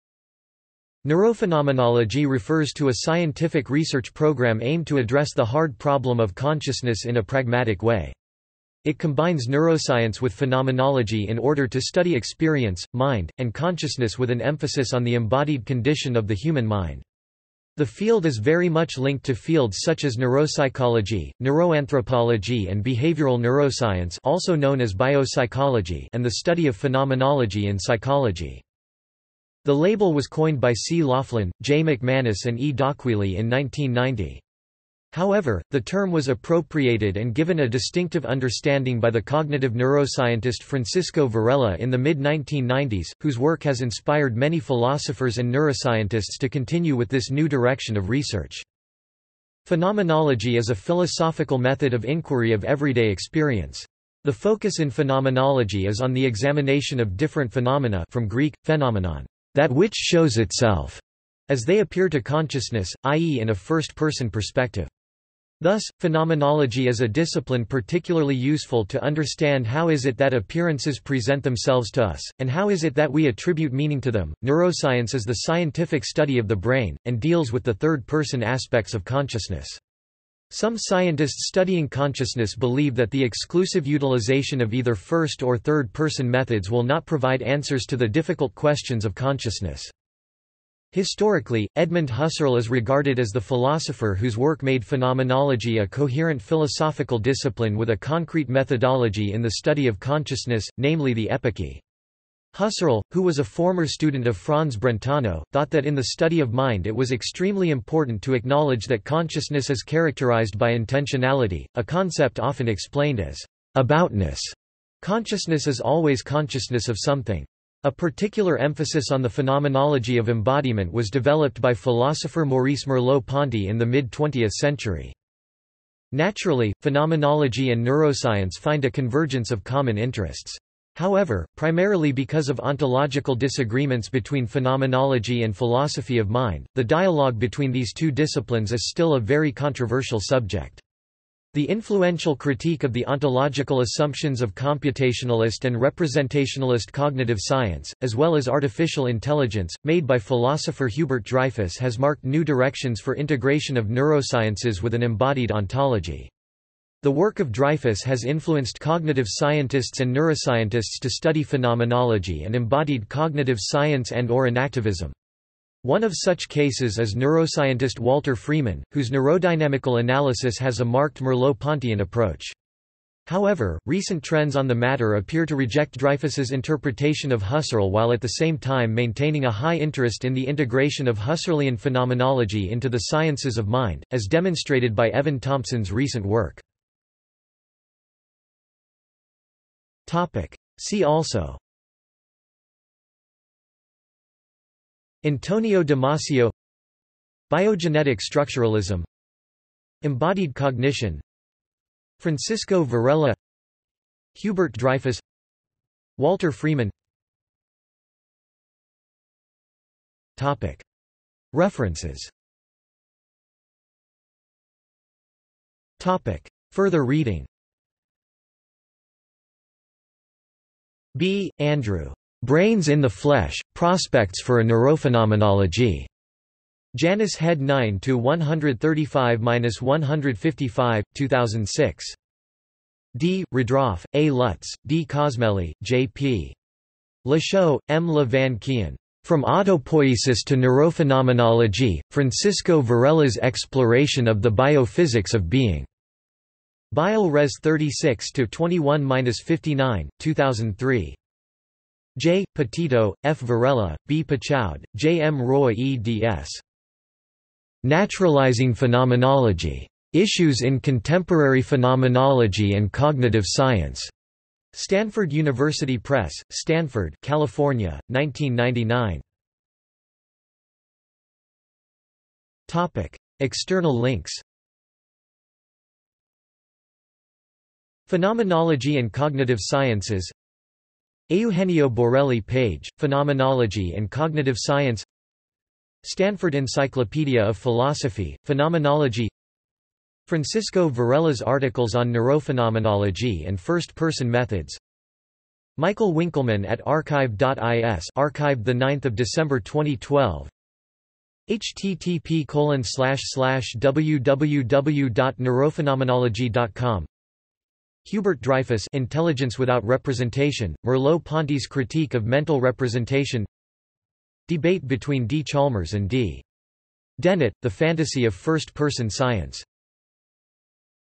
Neurophenomenology | Wikipedia audio article
This is an audio version of the Wikipedia Article:
Neurophenomenology

- Socrates

SUMMARY
Neurophenomenology refers to a scientific research program aimed to address the hard problem of consciousness in a pragmatic way. It combines neuroscience with phenomenology in order to study experience, mind, and consciousness with an emphasis on the embodied condition of the human mind. The field is very much linked to fields such as neuropsychology, neuroanthropology and behavioral neuroscience (also known as biopsychology) and the study of phenomenology in psychology.
The label was coined by C. Laughlin, J. McManus and E. d'Aquili in 1990. However, the term was appropriated and given a distinctive understanding by the cognitive neuroscientist Francisco Varela in the mid-1990s, whose work has inspired many philosophers and neuroscientists to continue with this new direction of research.
Phenomenology is a philosophical method of inquiry of everyday experience. The focus in phenomenology is on the examination of different phenomena (from Greek, phainomenon, "that which shows itself") as they appear to consciousness, i.e. in a first-person perspective. Thus, phenomenology is a discipline particularly useful to understand how is it that appearances present themselves to us, and how is it that we attribute meaning to them.Neuroscience is the scientific study of the brain, and deals with the third-person aspects of consciousness. Some scientists studying consciousness believe that the exclusive utilization of either first- or third-person methods will not provide answers to the difficult questions of consciousness.
Historically, Edmund Husserl is regarded as the philosopher whose work made phenomenology a coherent philosophical discipline with a concrete methodology in the study of consciousness, namely the epoche. Husserl, who was a former student of Franz Brentano, thought that in the study of mind it was extremely important to acknowledge that consciousness is characterized by intentionality, a concept often explained as "aboutness"; consciousness is always consciousness of something. A particular emphasis on the phenomenology of embodiment was developed by philosopher Maurice Merleau-Ponty in the mid-20th century.
Naturally, phenomenology and neuroscience find a convergence of common interests. However, primarily because of ontological disagreements between phenomenology and philosophy of mind, the dialogue between these two disciplines is still a very controversial subject. The influential critique of the ontological assumptions of computationalist and representationalist cognitive science, as well as artificial intelligence, made by philosopher Hubert Dreyfus has marked new directions for integration of neurosciences with an embodied ontology. The work of Dreyfus has influenced cognitive scientists and neuroscientists to study phenomenology and embodied cognitive science and/or enactivism. One of such cases is neuroscientist Walter Freeman, whose neurodynamical analysis has a marked Merleau-Pontyian approach. However, recent trends on the matter appear to reject Dreyfus's interpretation of Husserl while at the same time maintaining a high interest in the integration of Husserlian phenomenology into the sciences of mind, as demonstrated by Evan Thompson's recent work.